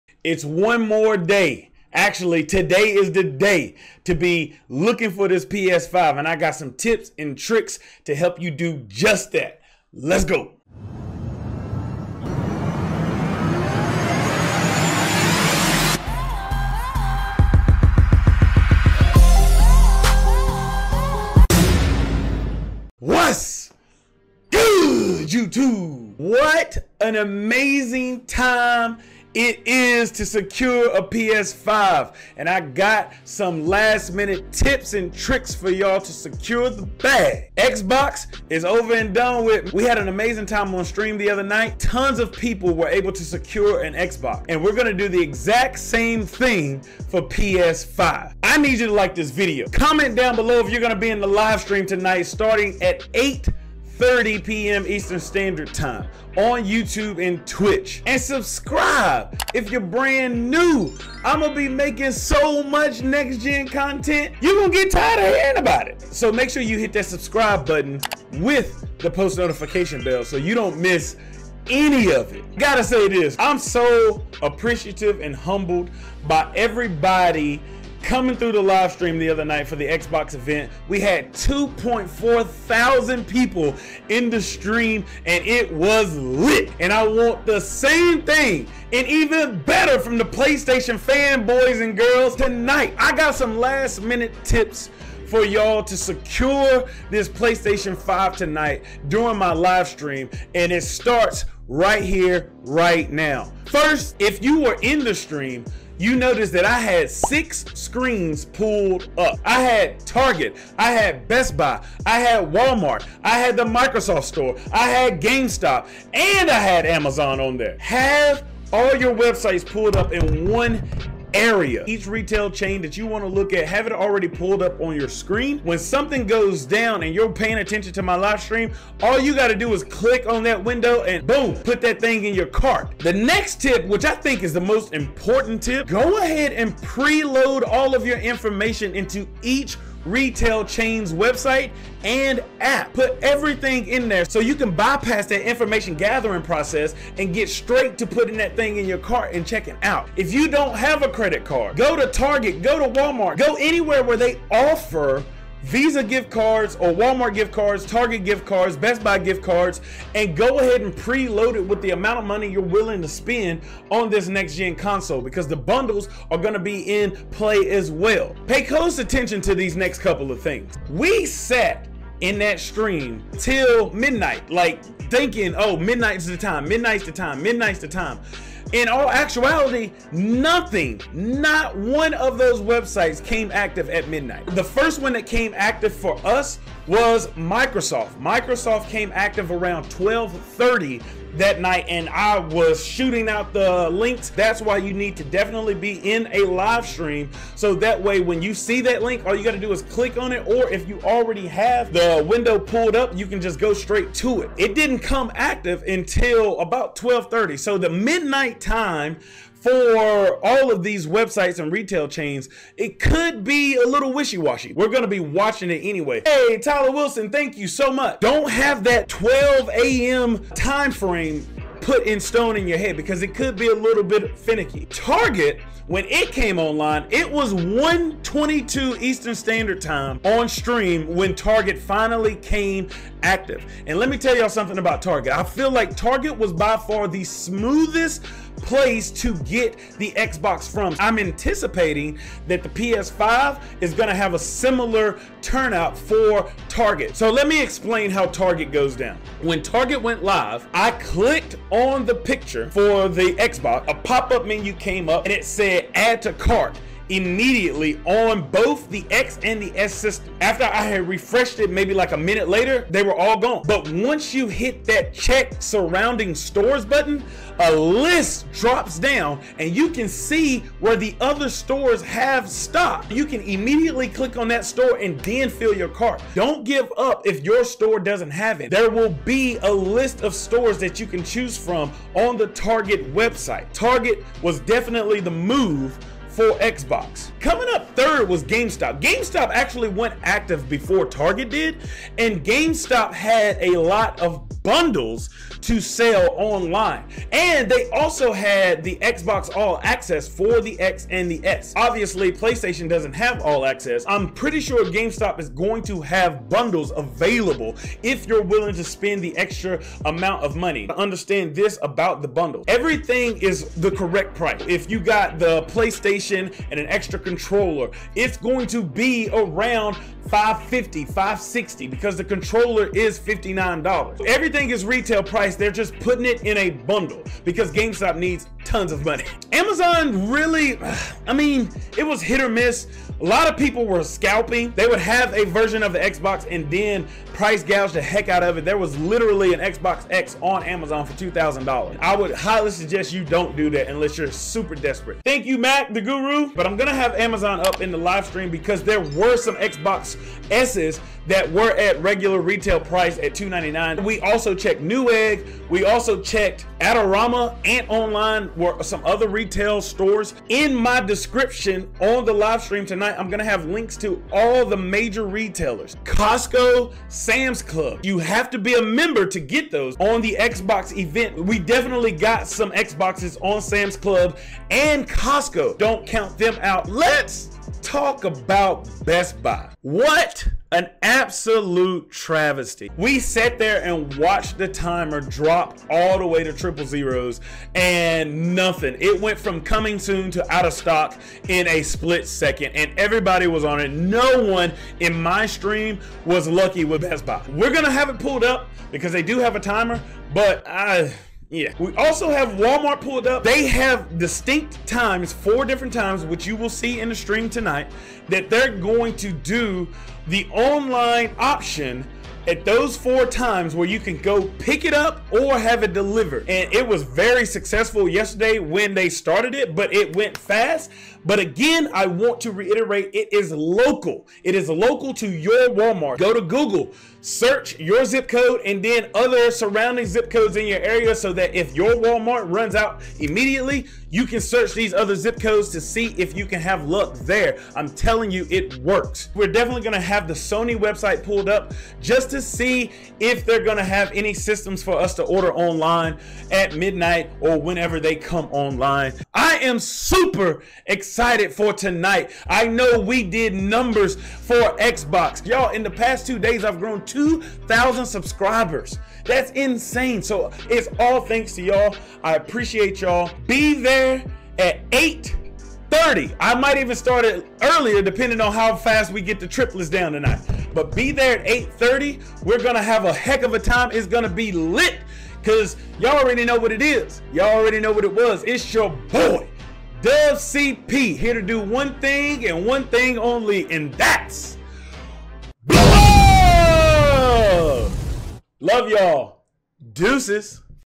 How to secure a PS5 on Launch Day | Last Minute Tips and Playstation 5 Updates
The moment is finally here for the release of the Playstation 5, and I have some last minute tips on how to secure a PS5 on Launch Day.  If you were not lucky enough to snag a PS5 Pre order, don't worry I got what you need in this lastest PS5 news video.